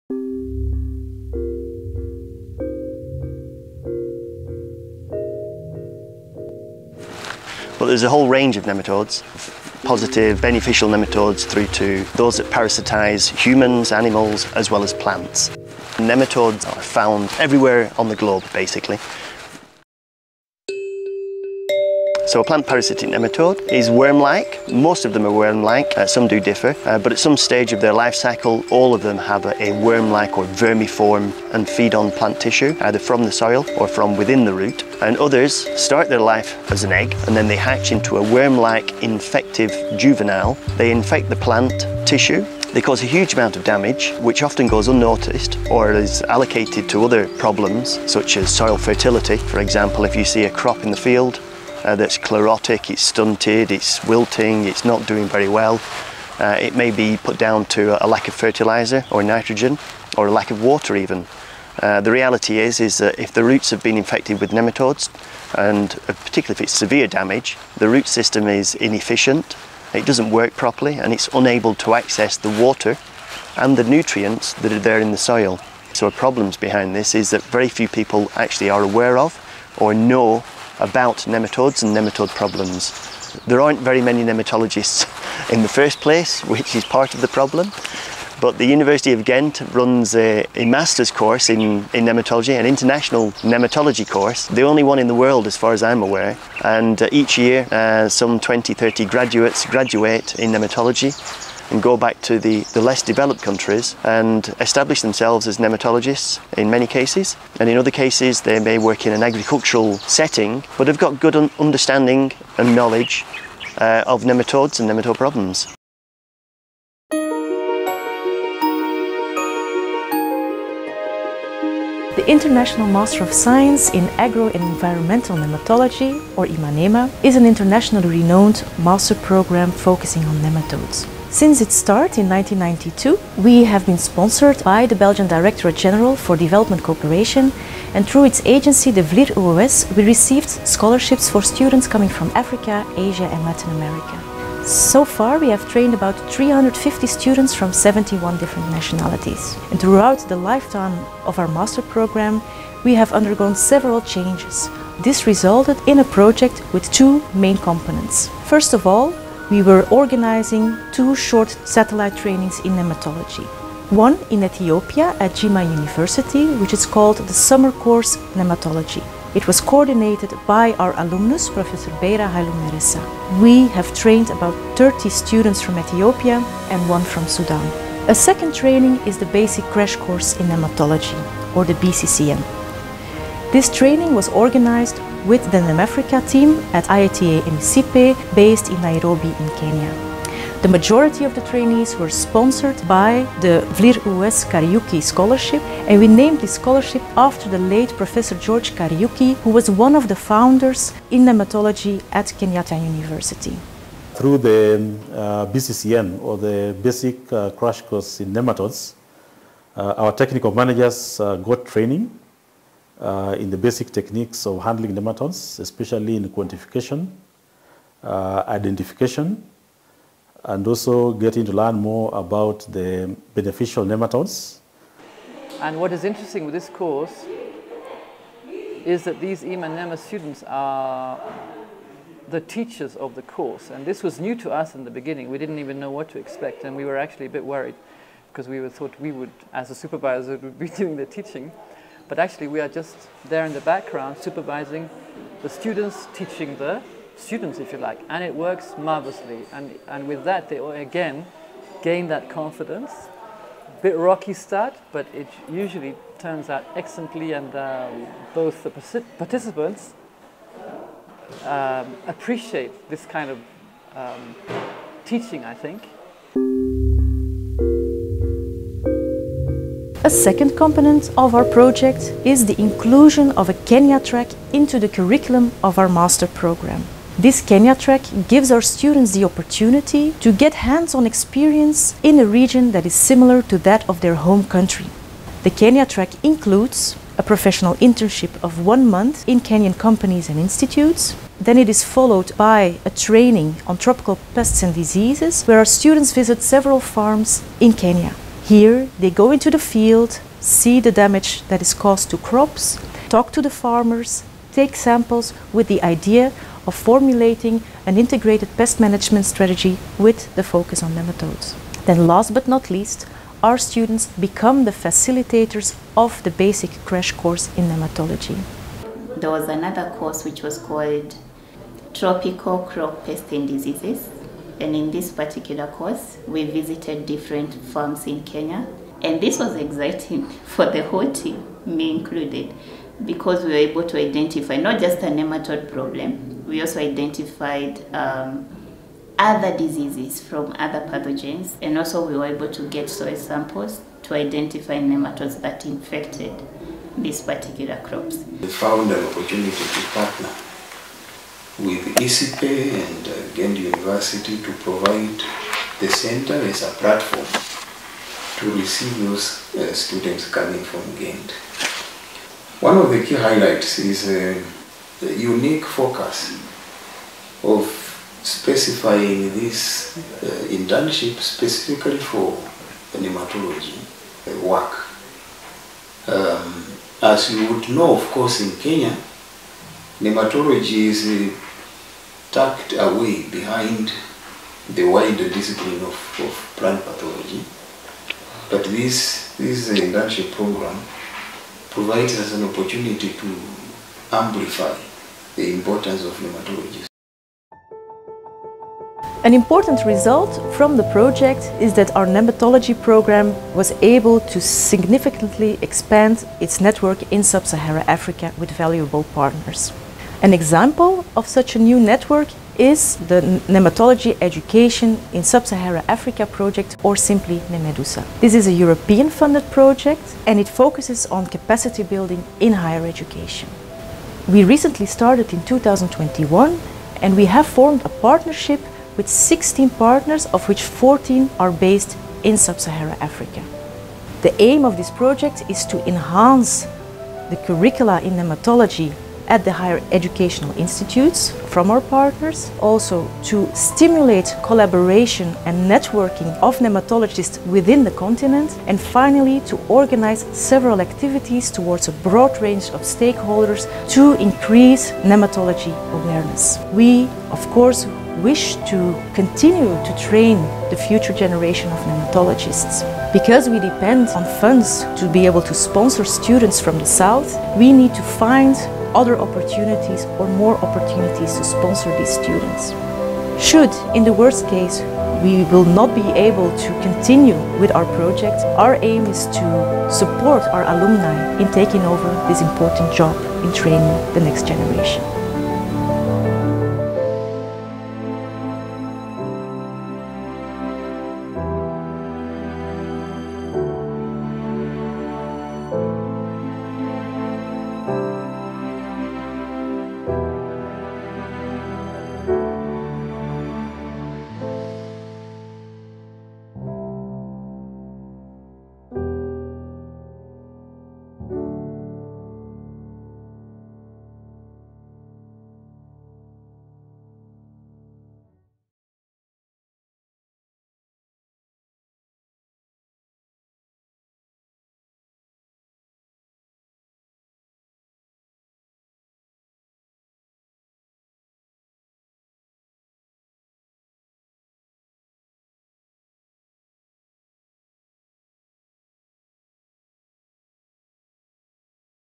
IMANEMA nematode projects in Africa
The International Master of Science in Agro- and Environmental Nematology (IMaNema) is a globally unique 2-year MSc programme in English and brings together internationally renowned staff to train students in the broad field of agricultural and environmental aspects of nematology. This video details what the project is about